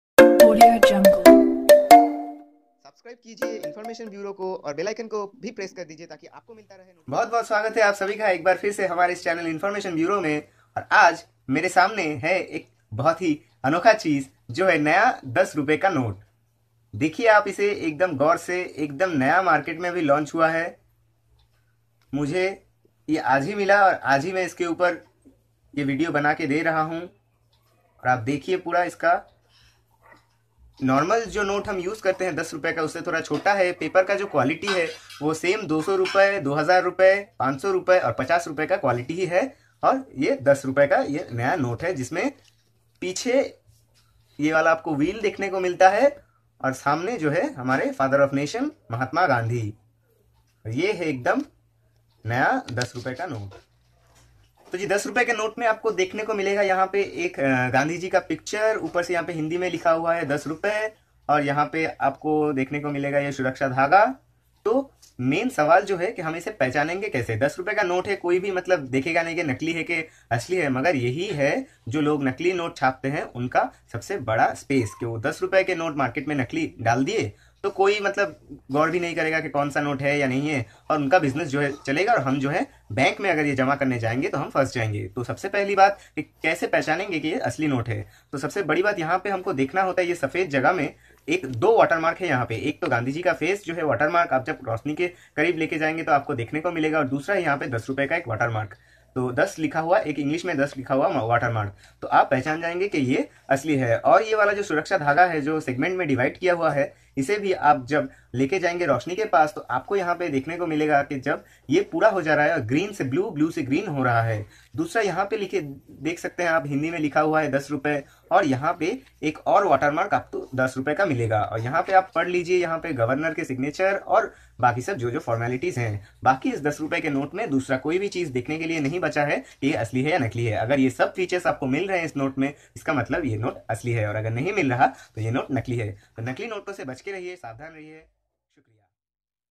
How to check New 10 Rupees Note ?? Real or Fake . Full explaination [In Hindi]
Indian Govt. Has launched new 10 rupees note for India as the base Currency , but it has a very Good risk of fake or copy or clone 10 rupees note since the amount is so low u make be cheated & no one is going to check that low amount whether it is Real or fake .
Don't be fool check it coz bank will not accept & the whole note will be Destroyed immediately & u will be legally punished.
Information Bureau
Pinaki Bagh